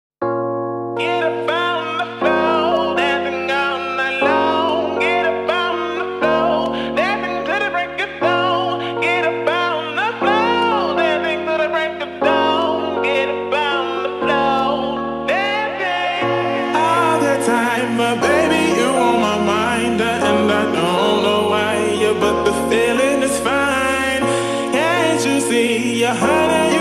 Amazing Drawing of Piccolo in 4 Different Styles!!
You have all spoken in my latest poll and you want to see Piccolo in 4 different styles, so here's part 1!!
we return to the origins of piccolo and keep it simple. what do your think?